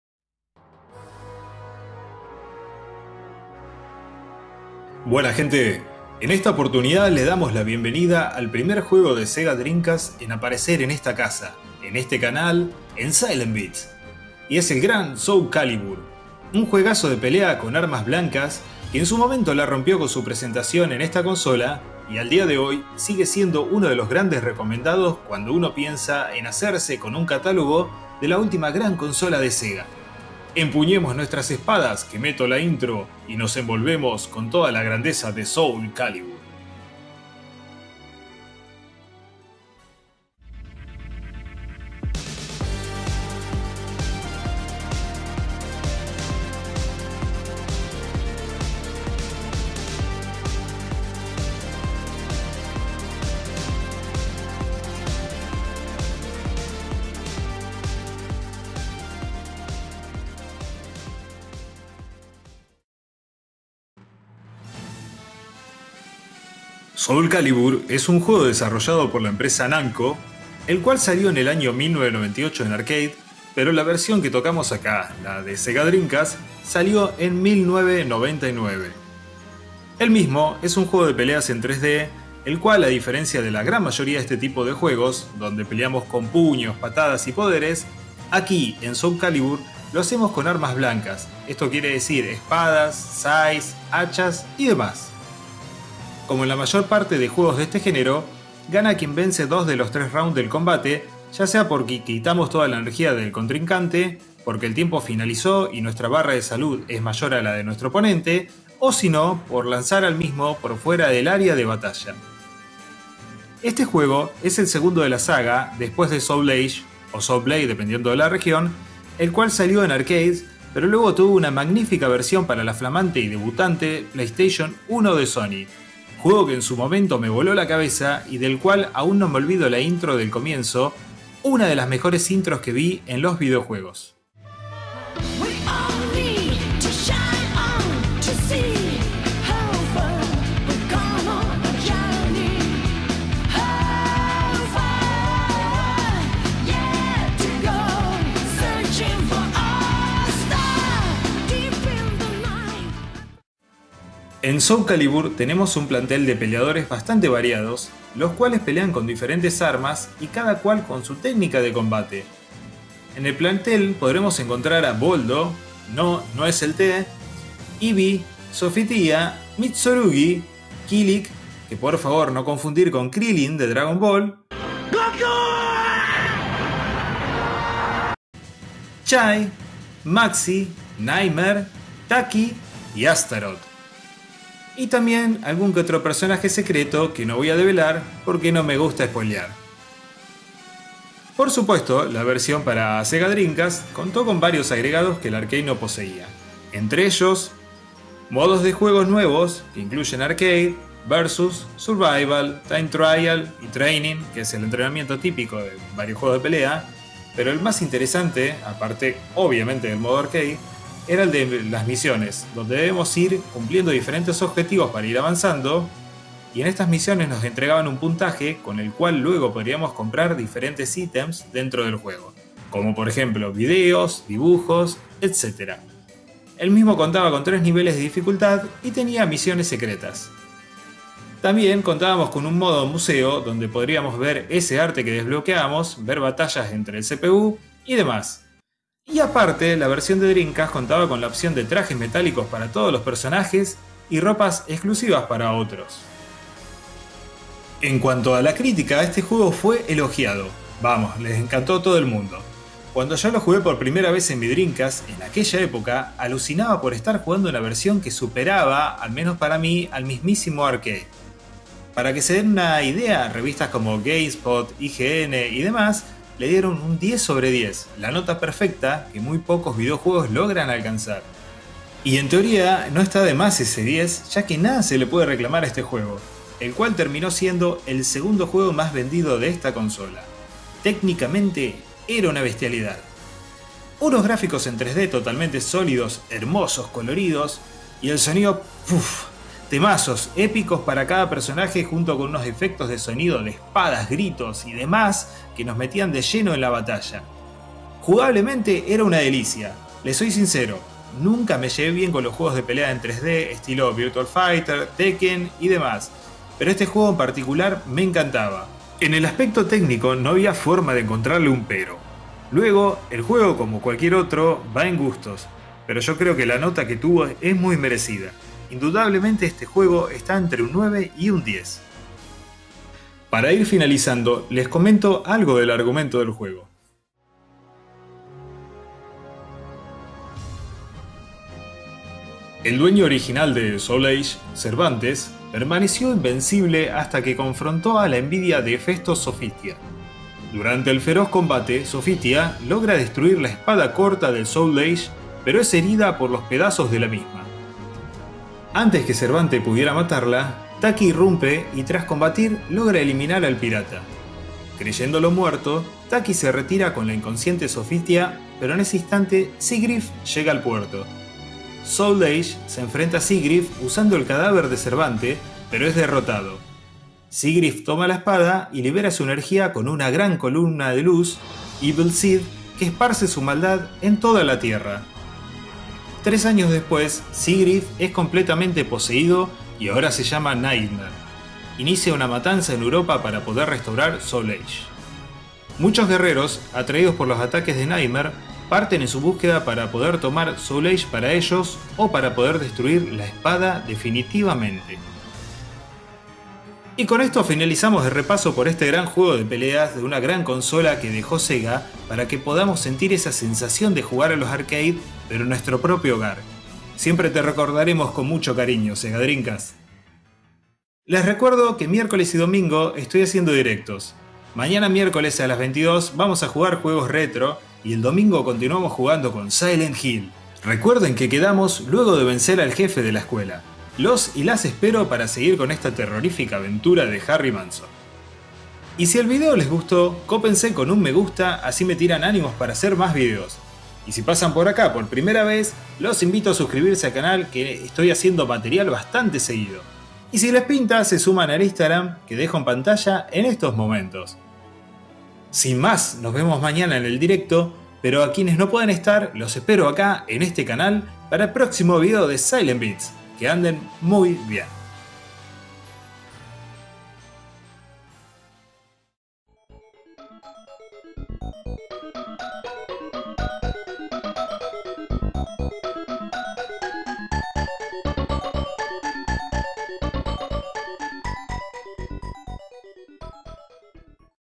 Hablemos de...SOUL CALIBUR - Sega Dreamcast!
Buenas gente!

Hoy repasamos uno de los mas grandes titulos de pelea,y de la sega dreamcast,Soul calibur.

Los/las invito a ver el video y comentar sus experiencias con el mismo en la caja de comentarios.

Que anden muy bien! =)
DIRECTOS:
*MIERCOLES Y DOMINGOS - DE 22:30 A 24:30 HS  (ARGENTINA).